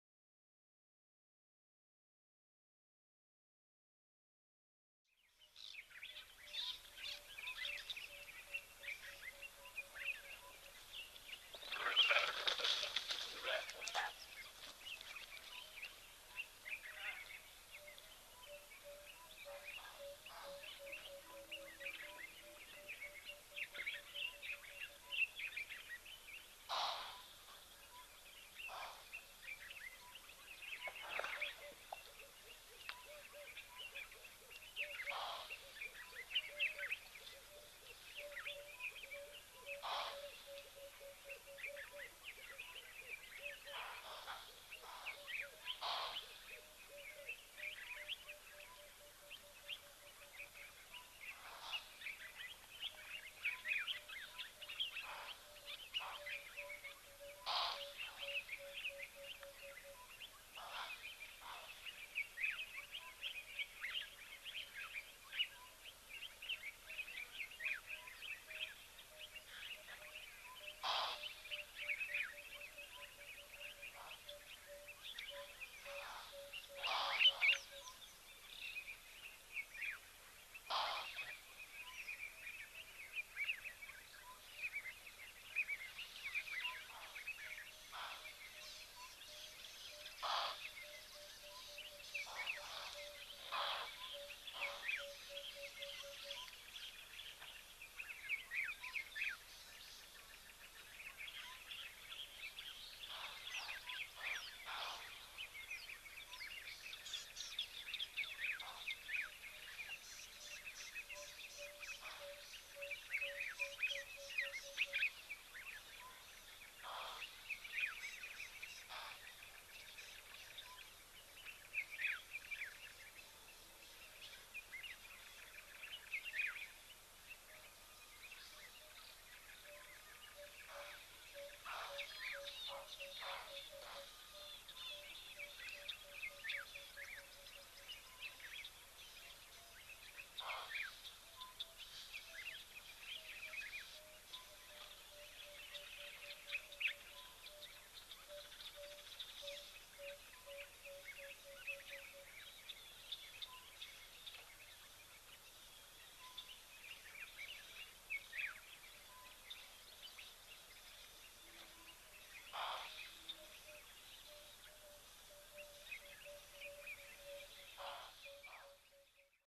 Lioness makes a Kill, a Nyala at Tembe
Tembe Lodge Hide at “Masahlela” Waterhole Cam,  KwaZulu-Natal, South Africa.
Tembe Elephant Reserve
Permission has been granted to make these videos by Africam.